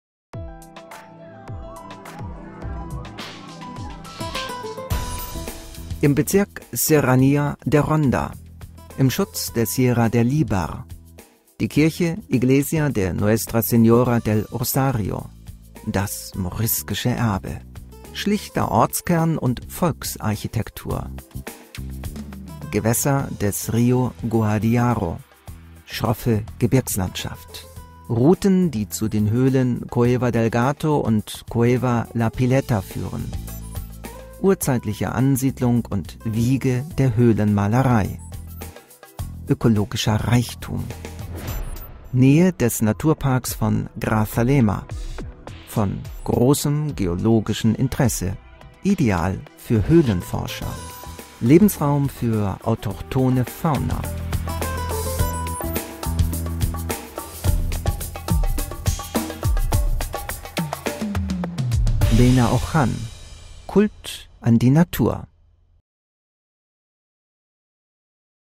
Benaoján HD: Ideal für Höhlenforscher. Provinz Málaga und Costa del Sol
Wanderstrecken zu den Höhlen CuevasdelGato und Cueva de la Pileta. Der Ort, der an den Naturpark Grazalema erinnert, besitzt einen großen Naturreichtum und geologische Bedeutung. Ideal für Höhlenforscher.